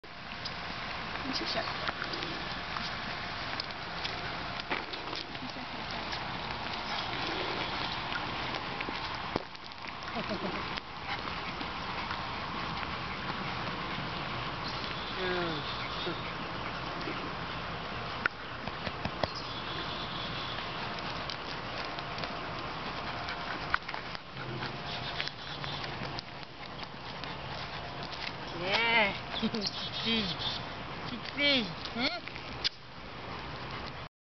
Jument miniature 2an et demi & mon chien Ranch BBSM
Note jument miniature adulte n'est pas plus grosse que notre chien! Elle est magnifique et adorable... sans oublier notre fidèle compagnon de vie qui adore toutes nos chevaux miniatures et en particulier les nouveaux nés!!!